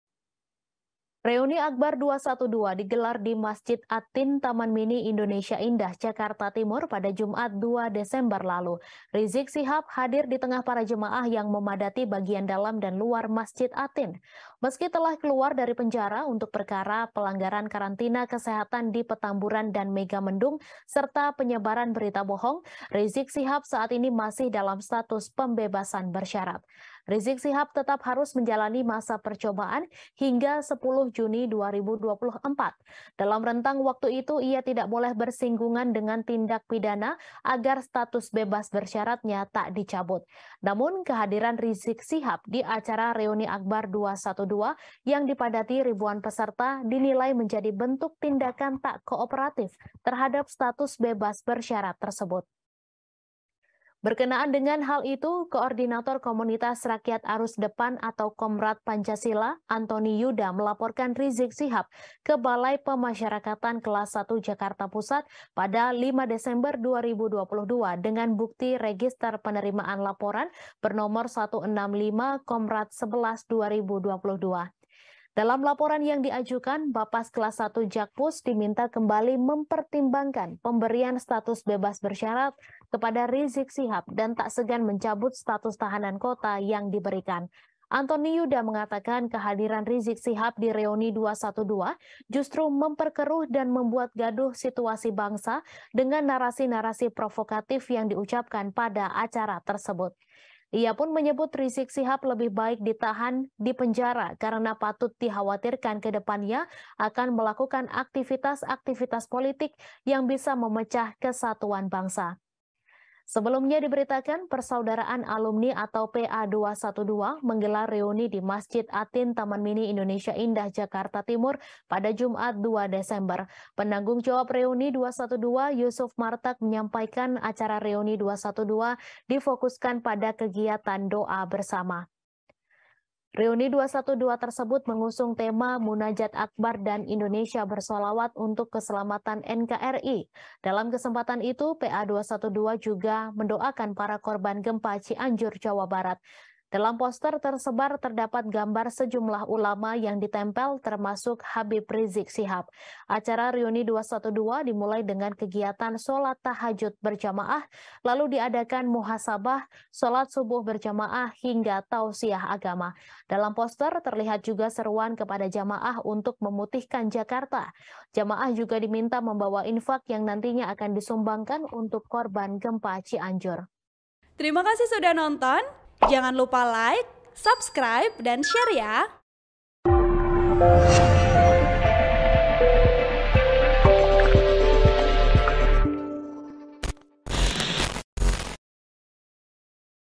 Kehadiran Rizieq Shihab di Reuni 212 Diadukan ke Bapas, Dinilai Buat Gaduh dengan Narasi Provokatif
TRIBUN-VIDEO.COM - Reuni Akbar 212 digelar di Masjid At-Tin Taman Mini Indonesia Indah (TMII) Jakarta Timur, pada Jumat (2/12/2022) lalu.

Rizieq Shihab hadir di tengah para jemaah yang memadati bagian dalam dan luar masjid At-Tin.

Meski telah keluar dari penjara untuk perkara pelanggaran karantina kesehatan di Petamburan dan Megamendung, serta penyebaran berita bohong, Rizieq Shihab saat ini masih dalam status pembebasan bersyarat (PB).

Rizieq Shihab tetap harus menjalani masa percobaan hingga 10 Juni 2024.

Dalam rentang waktu itu, ia tidak boleh bersinggungan dengan tindak pidana supaya status bebas bersyaratnya tak dicabut.

Namun kehadiran Rizieq Shihab di acara Reuni Akbar 212 yang dipadati ribuan peserta dinilai jadi bentuk tindakan tak kooperatif terhadap status bebas bersyarat tersebut.

“Rizieq Shihab sepertinya tidak mematuhi status bebas bersyarat yang masih harus dijalani sebagai bentuk prosedur hukum akibat perbutatannya,” kata Koordinator Komunitas Rakyat Arus Depan (KOMRAD) Pancasila, Antony Yudha kepada wartawan, Senin (5/12/2022).

Berkenaan dengan itu, Koordinator Komunitas Rakyat Arus Depan (KOMRAD) Pancasila, Antony Yudha melaporkan Rizieq Shihab ke Balai Permasyarakatan (Bapas) Kelas 1 Jakarta Pusat pada 5 Desember 2022, dengan bukti register penerimaan laporan bernomor 165/Komrad/XI/2022.

Dalam laporan yang diajukan, Bapas Kelas 1 Jakpus diminta kembali mempertimbangkan pemberian status bebas bersyarat kepada Rizieq Shihab, dan tak segan mencabut status tahanan kota yang diberikan.

Antony Yudha mengatakan, kehadiran Rizieq Shihab di reuni 212 justru memperkeruh dan membuat gaduh situasi bangsa dengan narasi-narasi provokatif yang diucapkan pada acara tersebut.

“Kehadiran dirinya di reuni 212 justru memperkeruh dan membuat gaduh situasi bangsa hari ini dengan narasi-narasi provokatif yang diucapkan pada acara tersebut,” ungkap dia.

Ia pun menyebut Rizieq Shihab lebih baik ditahan di penjara karena patut dikhawatirkan ke depannya akan melakukan aktivitas-aktivitas politik yang bisa memecah kesatuan bangsa.

“Kemenkumham dan Bapas Kelas 1 Jakarta Pusat harus mencabut status bebas bersyarat kepada Rizieq Shihab agar yang bersangkutan lebih baik ditahan di penjara karena patut dikhawatirkan ke depannya akan melakukan aktivitas-aktivitas politik yang bisa memecah kesatuan bangsa,” tutup Antony.

Sebelumnya diberitakan, Persaudaraan Alumni (PA) 212 menggelar reuni di Masjid At-Tin, Taman Mini Indonesia Indah (TMII), Jakarta Timur, Jumat (2/12/2022).

Penanggung jawab Reuni 212, Yusuf Martak, menyampaikan acara Reuni 212 difokuskan pada kegiatan doa bersama.

"Kami tidak hanya mengandalkan perjuangan, tapi kami butuh juga kekuatan doa."

"Maka dari itu, acara diadakan di sini sesuai dengan manfaatnya, yaitu kami berdoa dan bermunajat," ungkapnya dalam keterangan tertulis, Kamis (1/12/2022), dikutip dari TribunJakarta.com.

Reuni 212 tersebut mengusung tema Munajat Akbar dan Indonesia Bersalawat untuk Keselamatan NKRI.

Dalam kesempatan ini, PA 212 juga akan mendoakan para korban gempa Cianjur, Jawa Barat.

Dalam poster yang tersebar, terdapat gambar sejumlah ulama yang ditempel termasuk Habib Rizieq Shihab.

Acara Reuni 212 dimulai dengan kegiatan salat Tahajud berjamaah.

Lalu diadakan muhasabah, salat Subuh berjamaah, hingga tausiyah agama.

Dalam poster, terlihat juga seruan kepada jamaah untuk memutihkan Jakarta.

Jamaah juga diminta membawa infaq yang nantinya akan disumbangkan untuk korban gempa Cianjur.(*)

Artikel ini telah tayang di Tribunnews.com dengan judul Berstatus Bebas Bersyarat, Kehadiran Rizieq Shihab di Reuni 212 Diadukan ke Bapas Jakpus,
Penulis: Danang Triatmojo